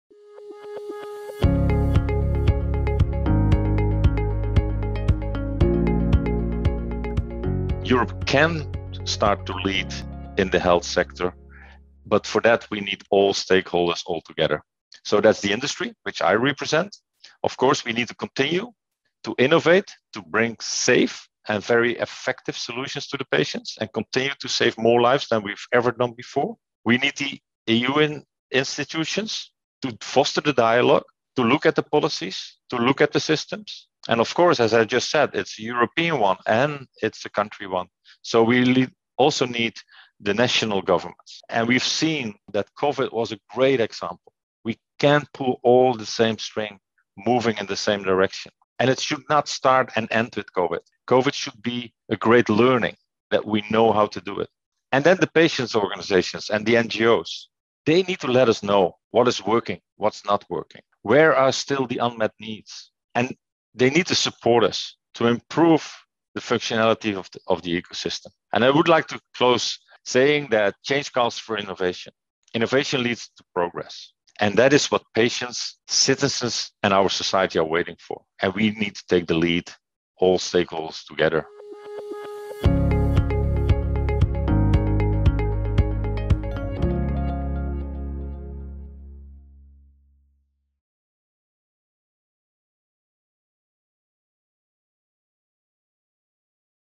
EUnite Week Session 3: Every stakeholder has a role to play in improving the healthcare sector
Kees Roks, Head of Region Europe at Novartis Oncology, highlights how all stakeholders can come together and work on improving healthcare in Europe for all by supporting Europe's role in the healthcare sector through innovation and progress.

EUnite Week occurred from 29 June to 1 July, 2021. The event series brought together a diverse group of stakeholders to discuss the future of European healthcare over three thematic sessions

Session 1: Sustainable Visions for health and wealth
Session 2: Harnessing the power of AI and big data for the future
Session 3: Innovating to deliver a healthcare ecosystem

EUnite Week was sponsored in partnership by: EFPIA, EuropaBio, the European Cancer Patient Coalition, InoMed, JA Europe, Novartis, ScanBalt, and the WifOR Institute.